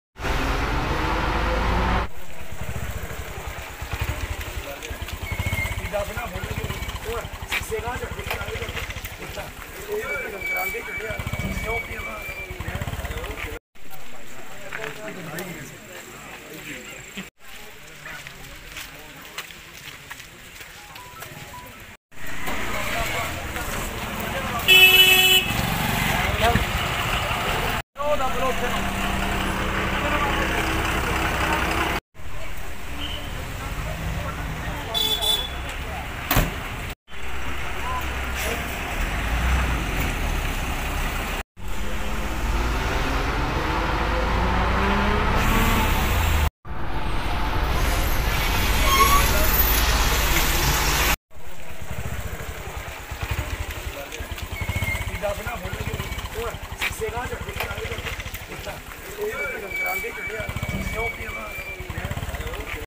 RSSB सत्संग घर रक्कड़ के नजदीक पास देते समय दो बसें टकराईं, लगा रहा लंबा जाम, मौके पर पहुंची पुलिस
Radha Soami Satsang Beas RAKKAR
BUS ACCIDENT
HRTC
RSSB
HAMIRPUR
PR NEWS